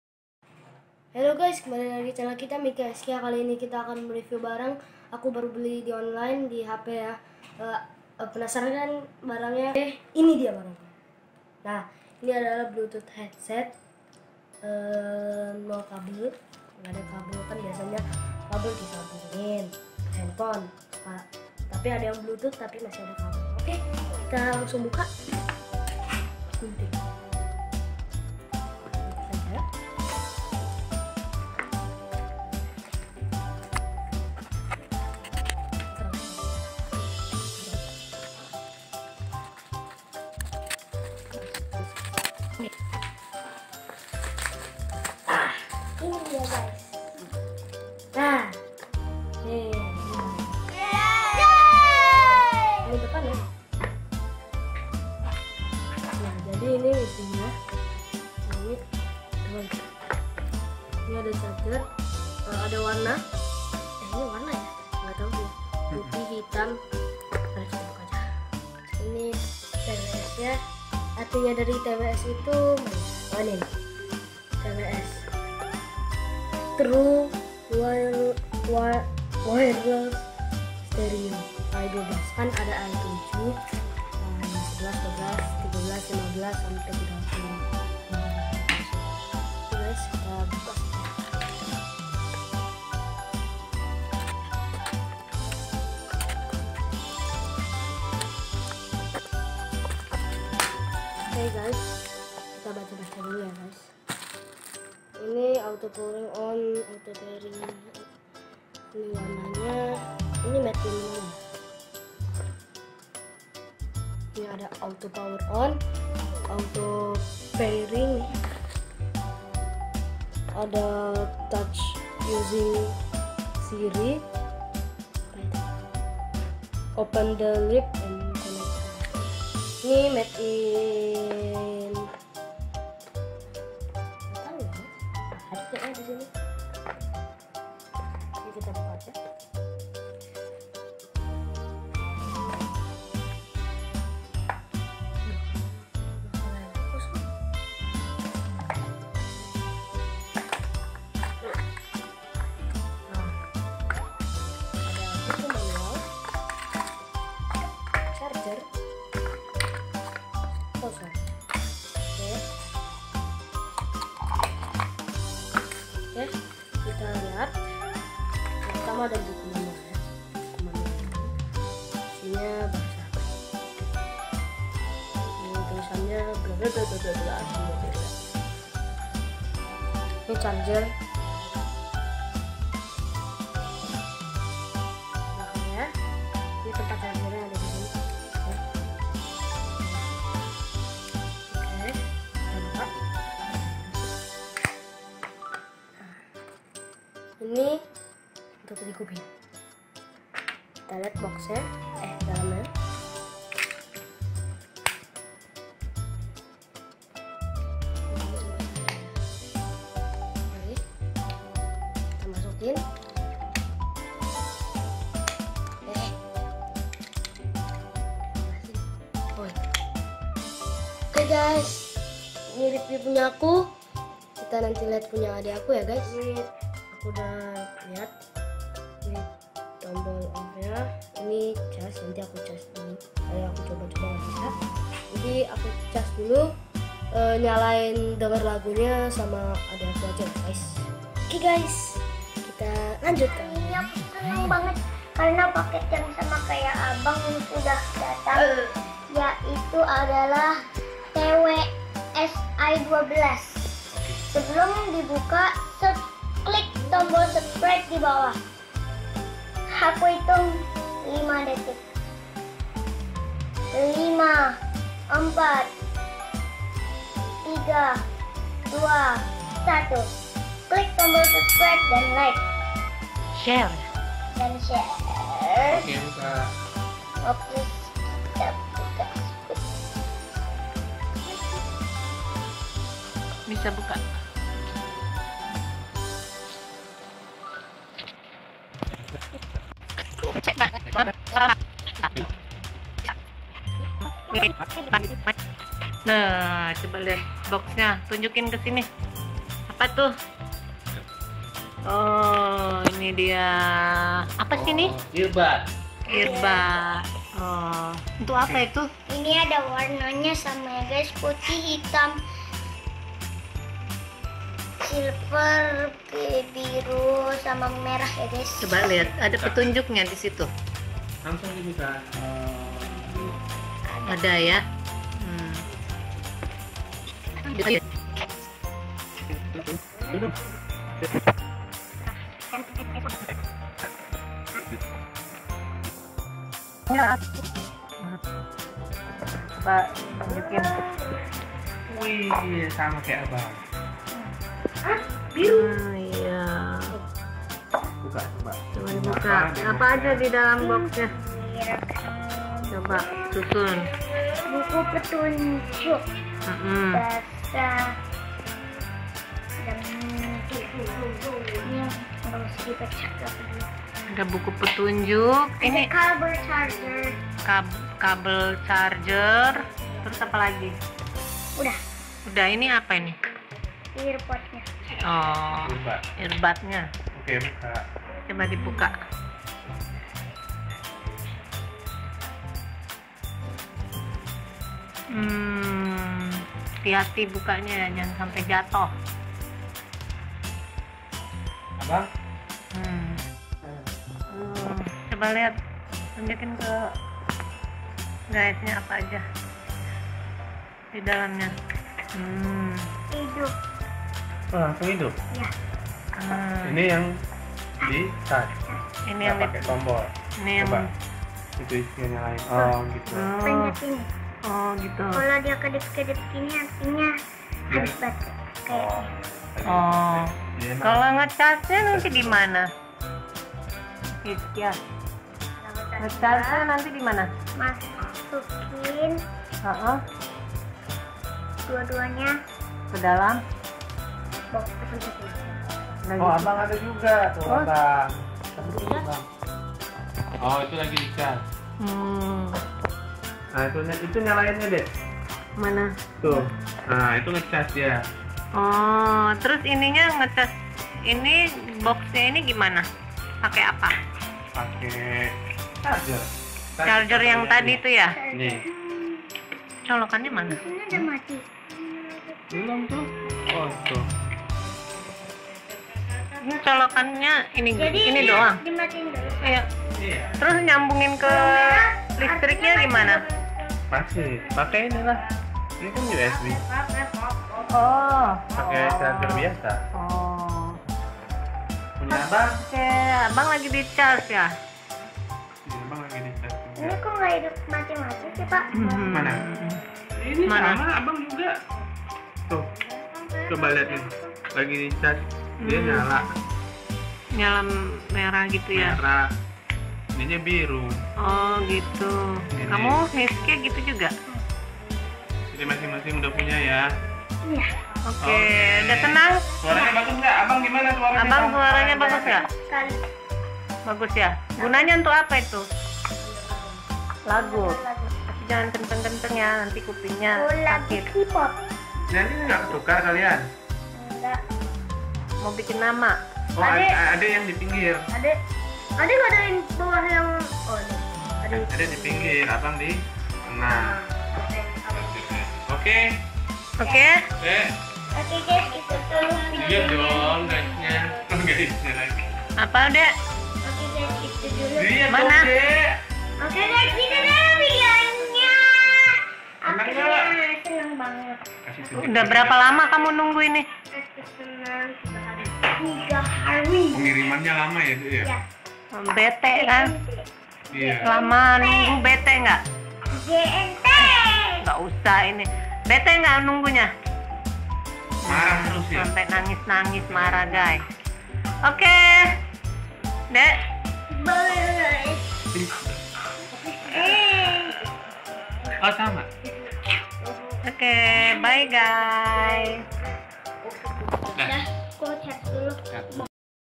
Unboxing Earbuds TWS i12 Mikha Hizkia
Guys, aku mau unboxing TWS i12. Dukung Channel aku ya guys.